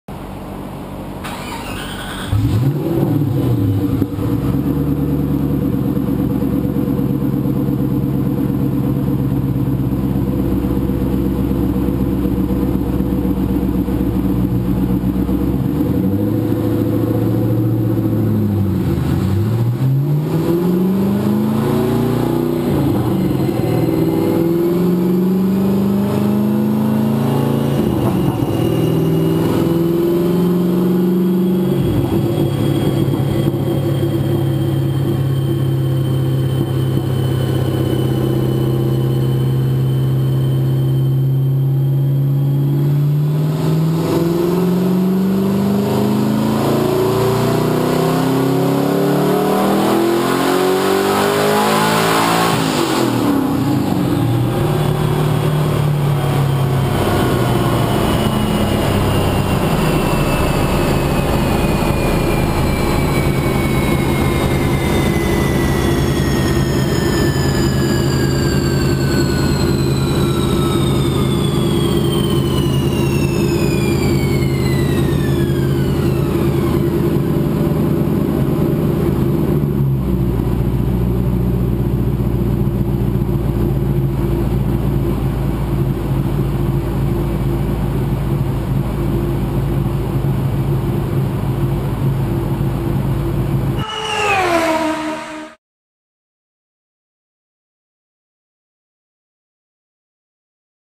Leaded vs. Unleaded Race Fuel
A test of VP C16 vs Shell URT Advanced unleaded race fuel